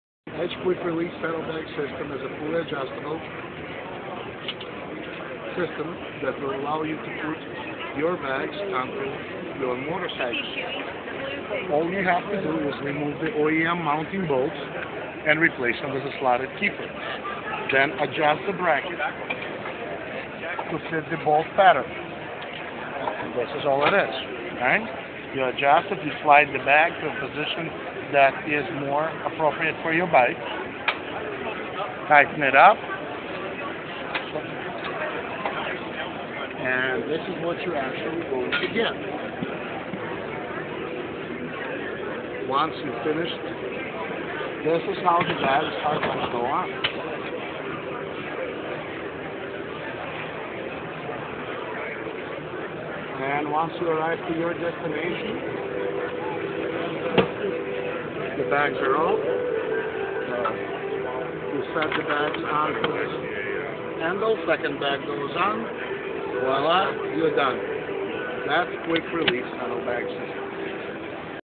Edge Brackets quick product demo.
Attach and Remove your motorcycle saddlebags in seconds.